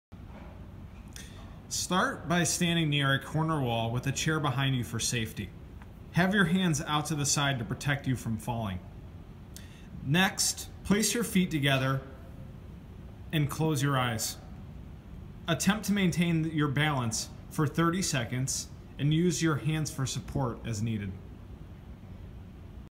Romberg Balance with Eyes Closed
Start by standing near a corner wall with a chair behind you for safety.  Have your hands out to the side to protect you from falling.

Place your feet together and close your eyes.  Attempt to maintain your balance for 30 seconds and use your hands for support as needed.